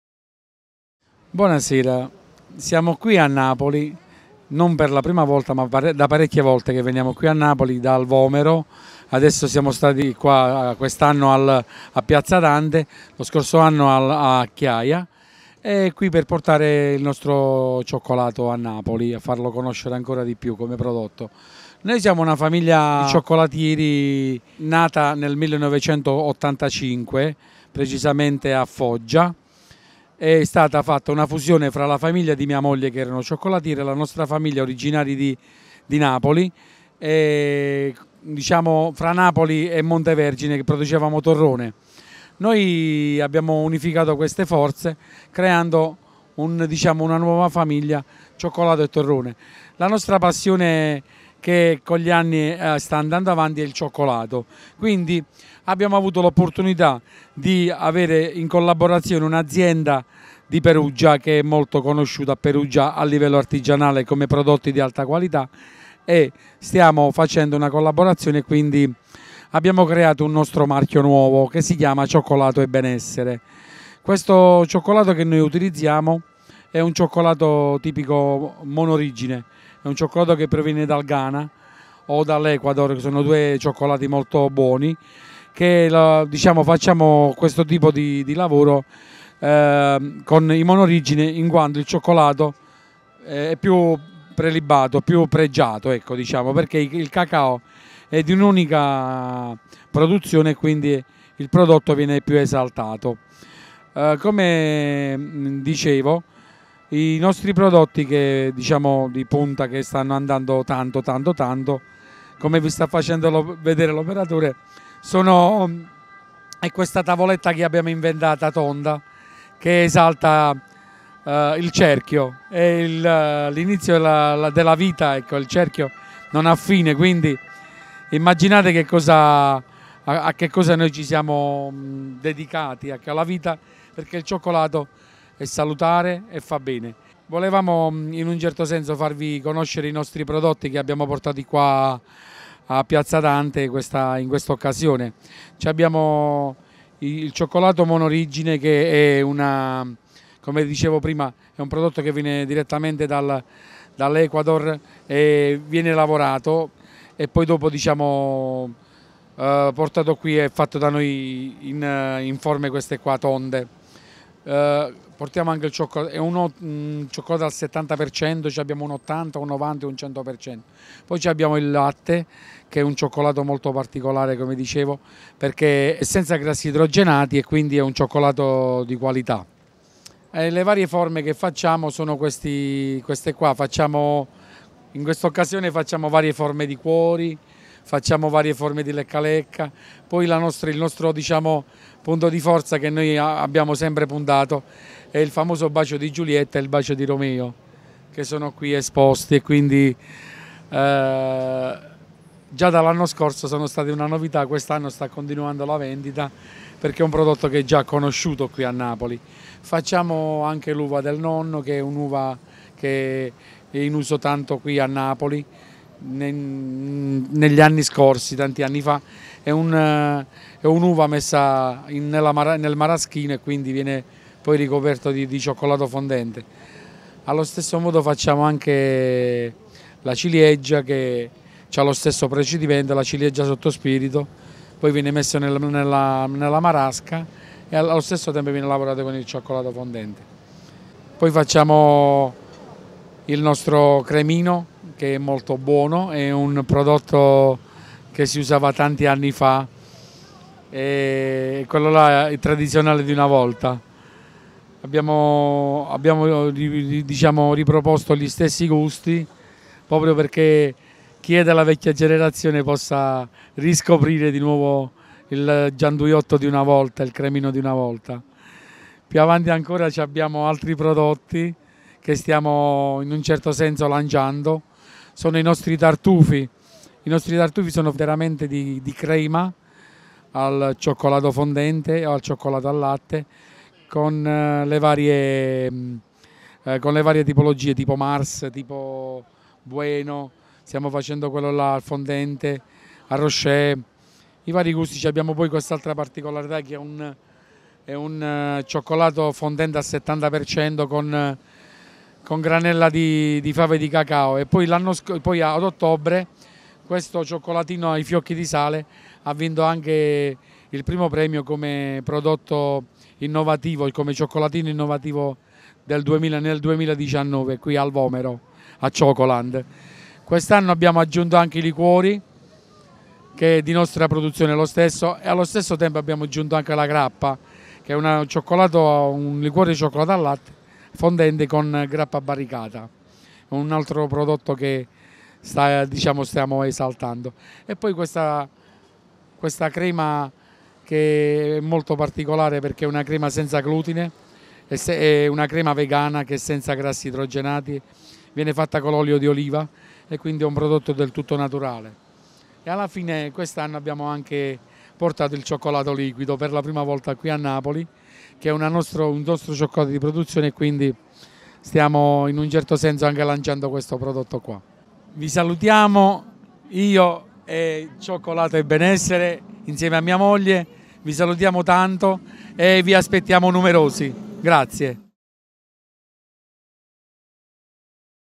il maestro Lorenzo Di Gennaro presentazione Cioccolato e Benessere
Il.maestro Lorenzo Di Gennaro ci presenta la sua azienda ed i suoi.prodotti